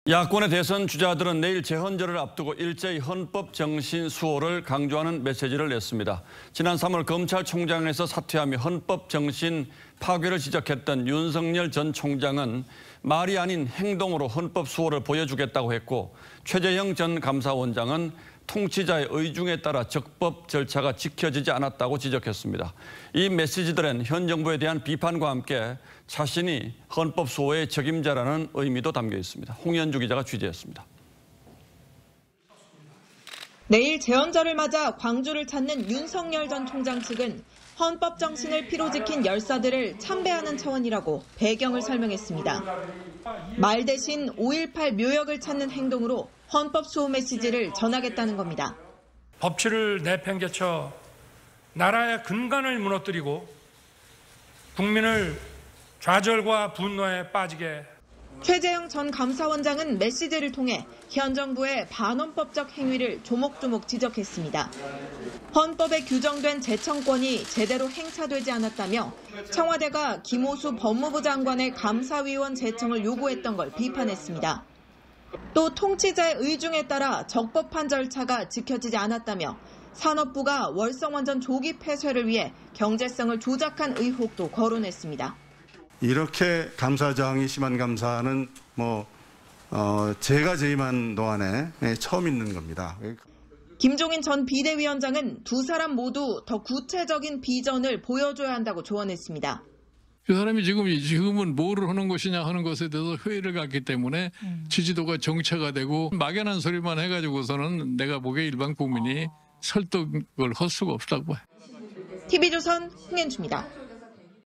윤석열 "헌법수호, 말 아닌 행동"…최재형 "통치자 의중 따라 적법절차 어겨" [뉴스9]
야권 대선주자들은 내일 제헌절을 앞두고 일제히 헌법정신 수호를 강조하는 메시지를 냈습니다. 지난 3월, 검찰총장에서 사퇴하며 헌법정신 파괴를 지적했던 윤석열 전 총장은 말이 아닌 행동으로 헌법수호를 보여주겠다고 했고, 최재형 전 감사원장은 "통치자의 의중에 따라 적법 절차가 지켜지지 않았다"고 지적했습니다. 이 메시지엔 현정부에 대한 비판과 함께 자신이 헌법수호의 적임자라는 의미도 담겨 있는 듯 합니다.